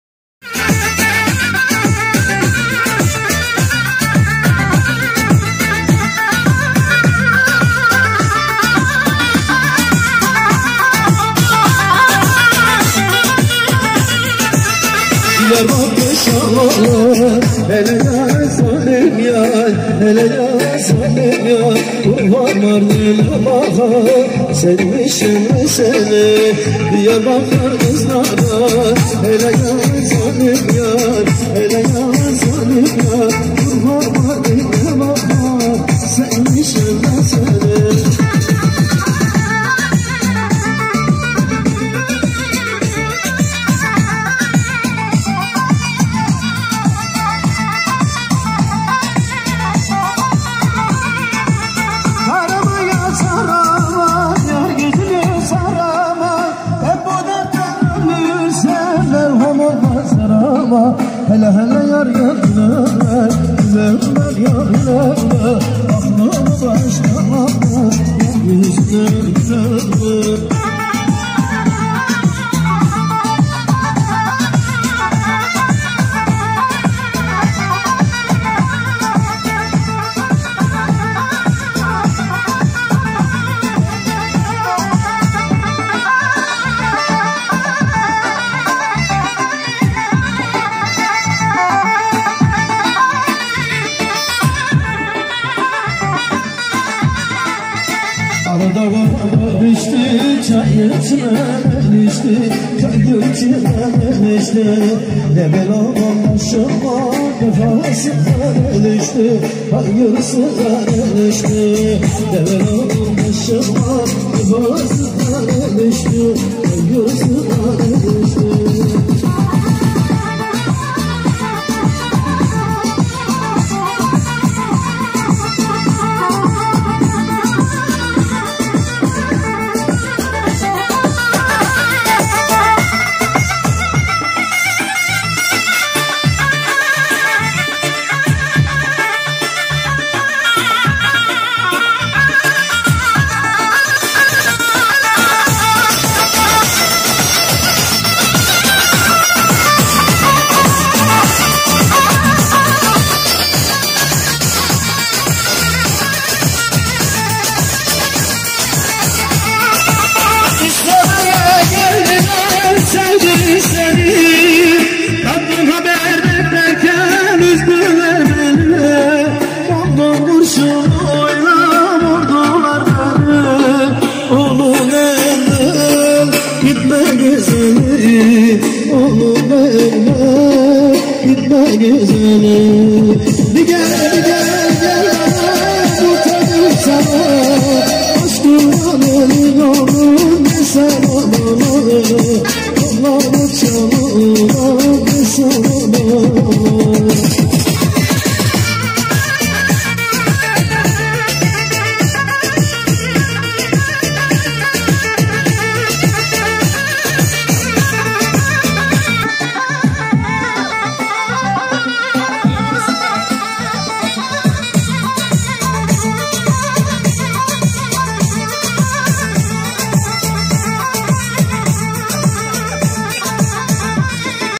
Bizimelin halayı istanbul esenyurt şükrü Sarıtepeci düğünü kırıkkale kız tarafı
Dügün#halay#anadolu#SUNGURLU#balkaya#karaoluk#yollarında#sungurlu#kırıkkale#konurkale#yarımsögüt#beşkızköyü#Haydar#ismail